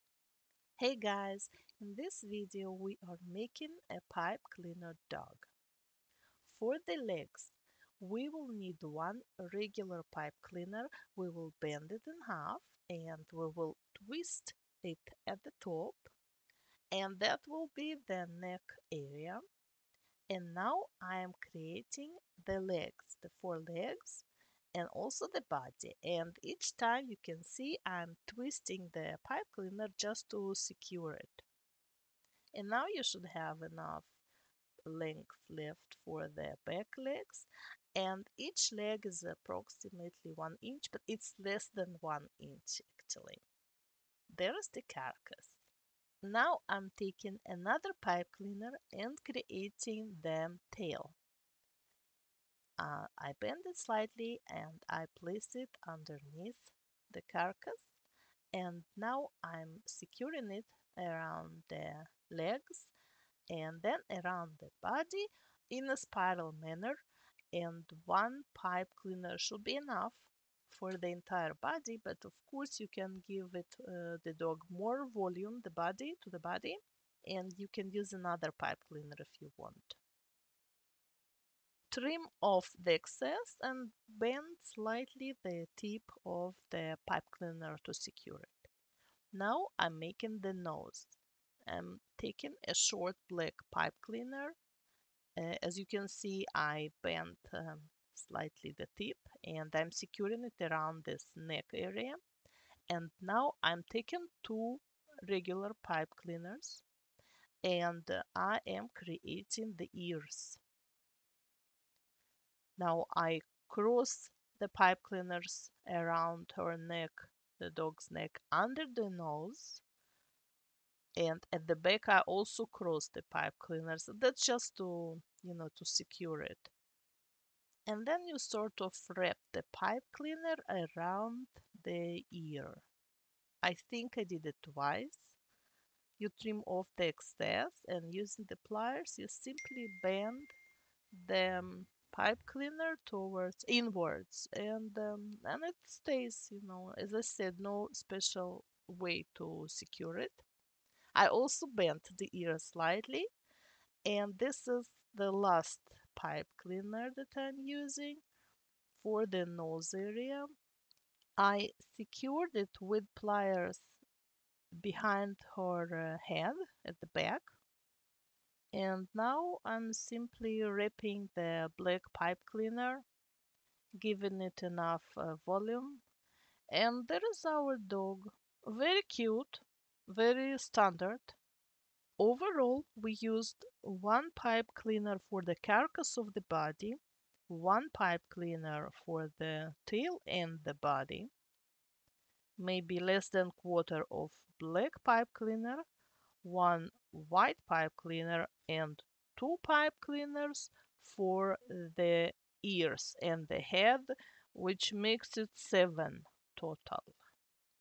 How to make a Pipe Cleaner Dog (no glue)
How to make a Pipe Cleaner Dog / pipe cleaner animals
pipe cleaner dog tutorial
you will need 7 pipe cleaners.
1 for the body carcass
1 for the tail and body
1/4 black - for the nose tip
2 for the ears and head
1 white or less - for the nose